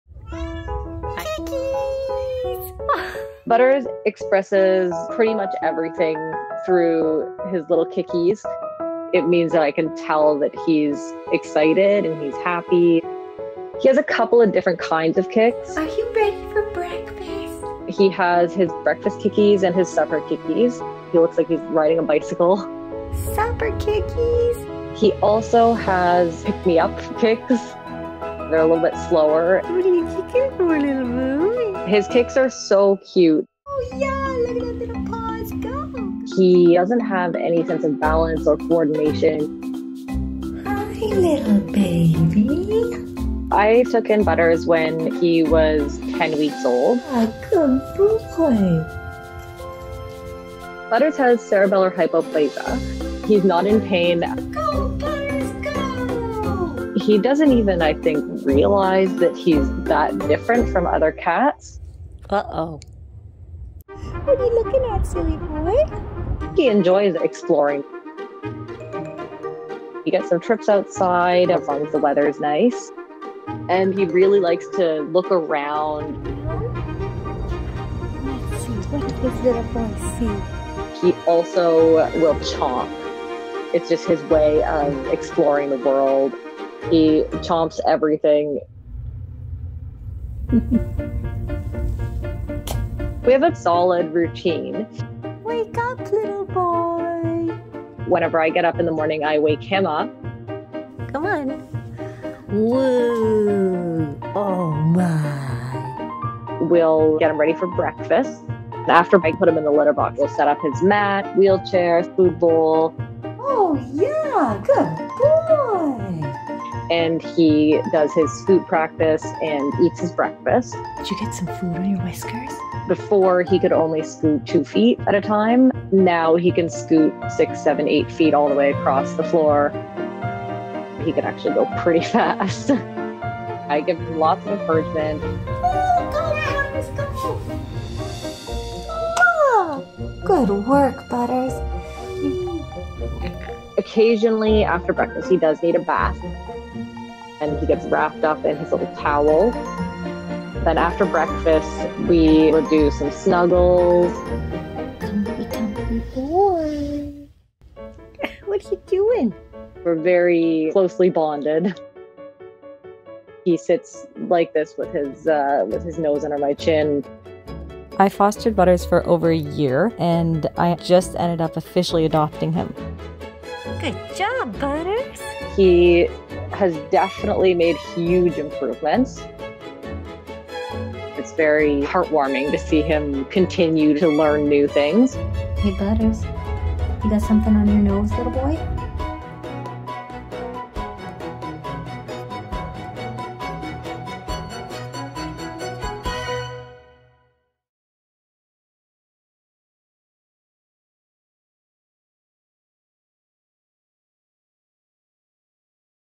Cat Does The Cutest Thing When He's Happy | The Dodo
Cat tells his mom he's happy in the cutest way 💕

Keep up with Butters on Instagram and TikTok Keep up with Team Cat Rescue on TikTok & Instagram